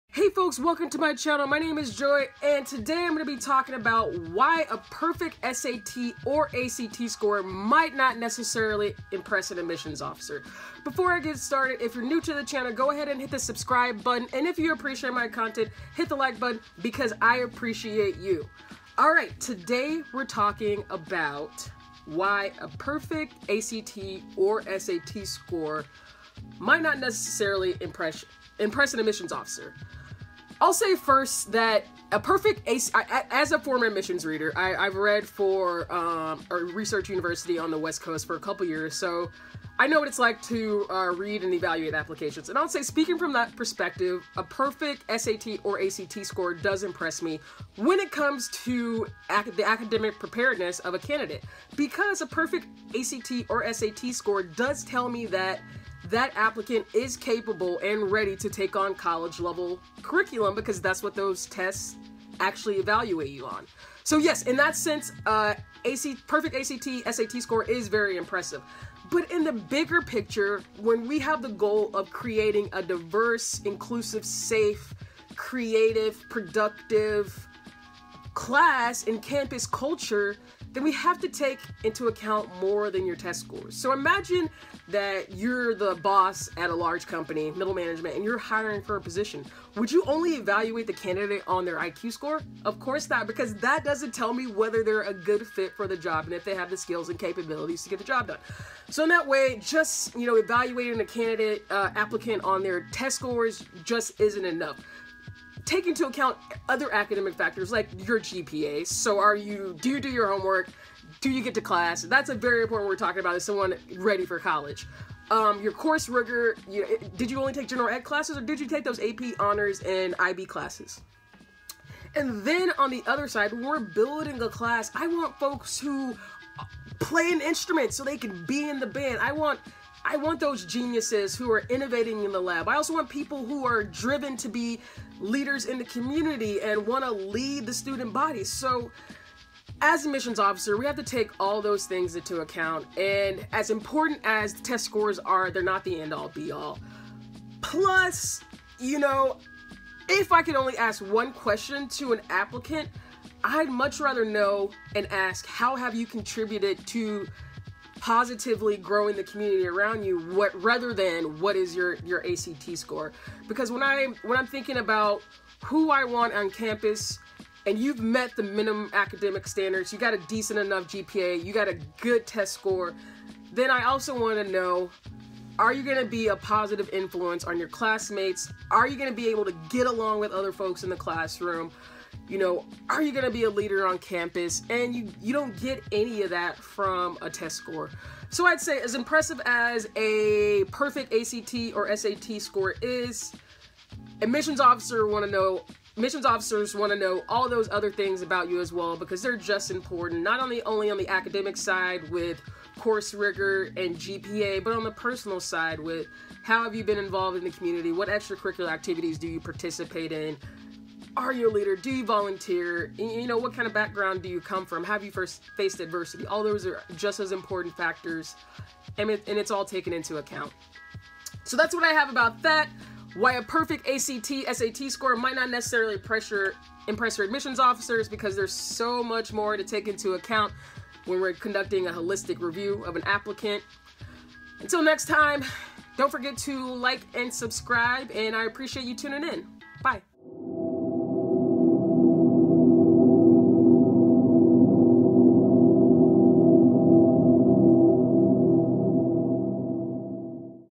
Why a Perfect ACT or SAT Score Might Not Impress Admissions Officers
A perfect SAT or ACT score is certainly an accomplishment...still it may not necessarily impress every admissions officer you run into. Here's why.

You can also check out my blog on writing personal statements and other college admissions-related info